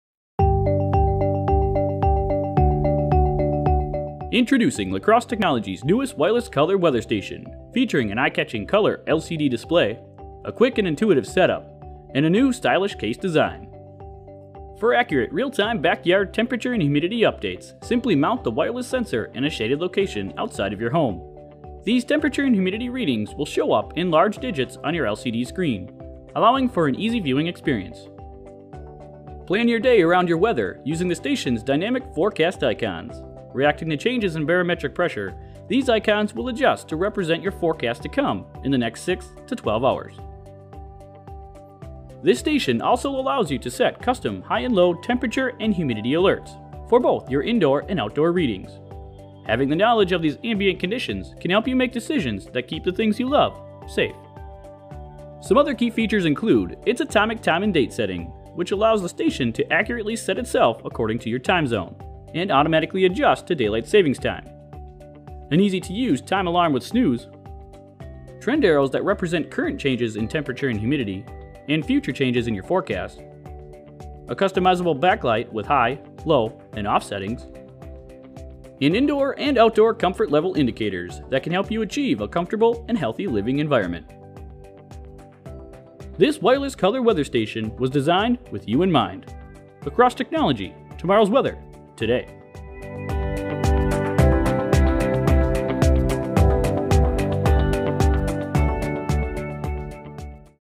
La Crosse Technology C85845-1 Color Wireless Forecast Station
Visit

Dynamic forecast icons with tendency arrows

Temperature & humidity (hi/lo) customizable alerts

Comfort level color bar

Adjustable backlight.Sensor dimension: 1.57 L x 0.79 W x 5.12 H inches

Self setting accurate atomic time and date with automatic daylight savings time reset (on/off optional)